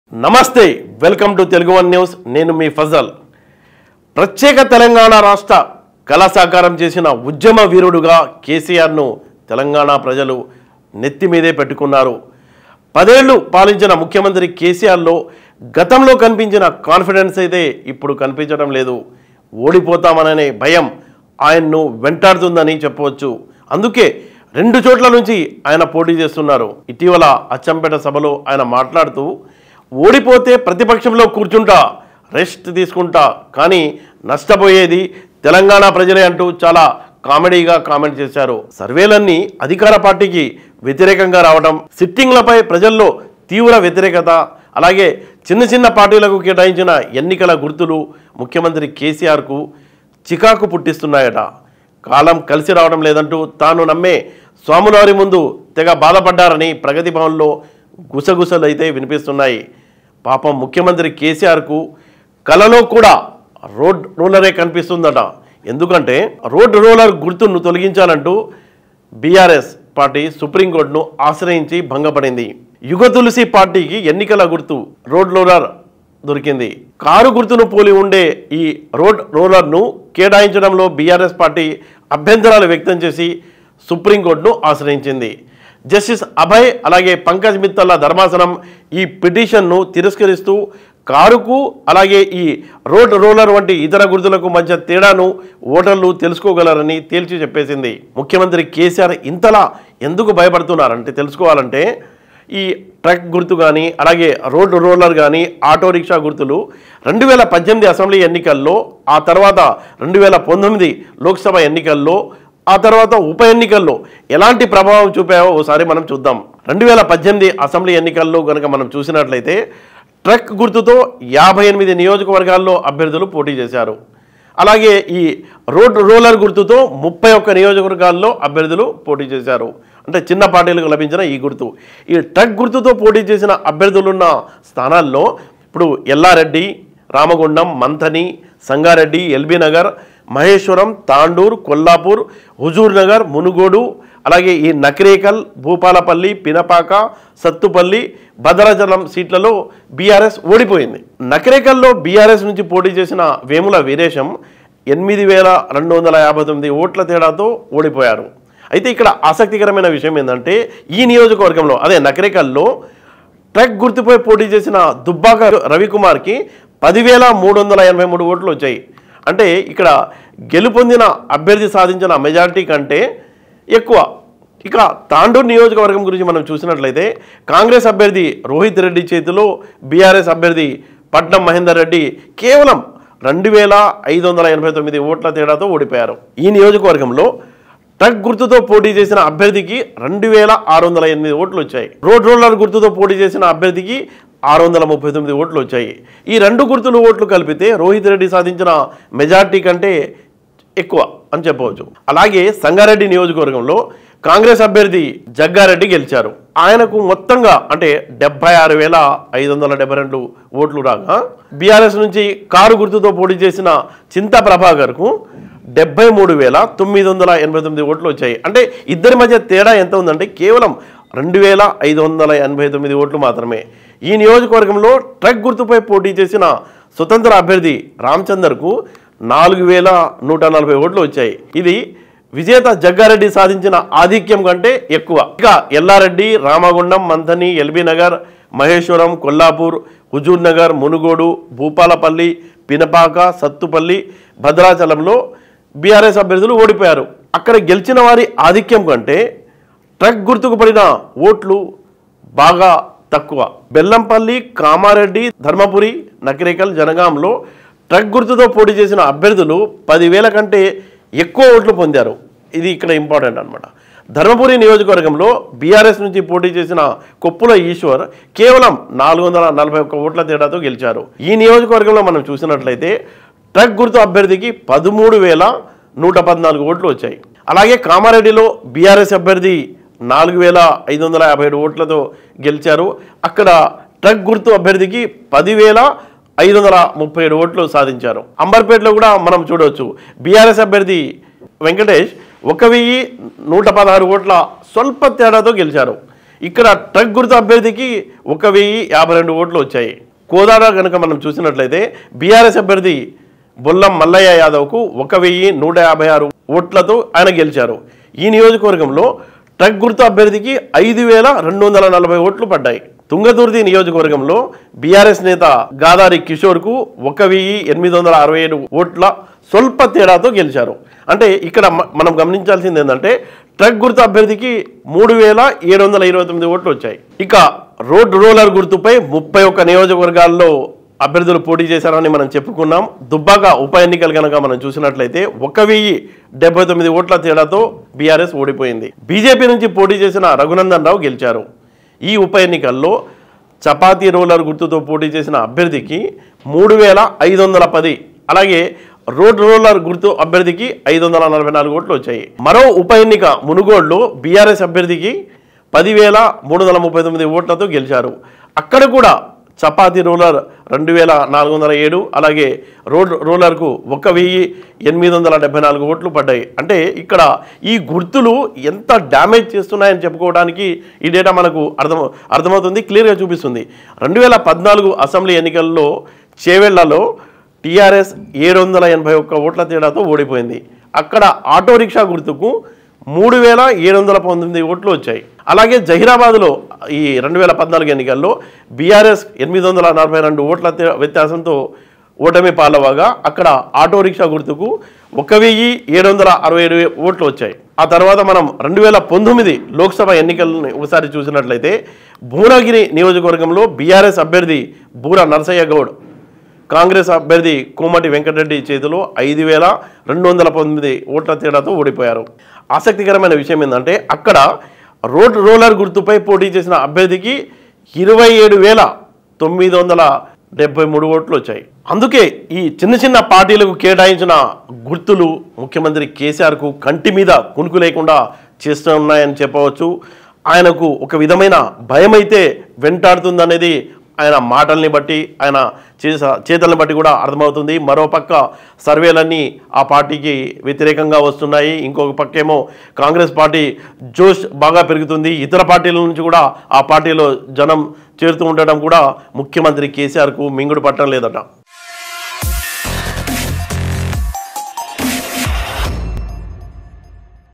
బెంగపెట్టుకున్న కేసీఆర్.! | Sr Journalist Fazal Analysis | KCR contesting in 2 constituencies | Tone
బెంగపెట్టుకున్న కేసీఆర్..!  Sr Journalist Fazal Analysis | KCR contesting in 2 constituencies | Tone News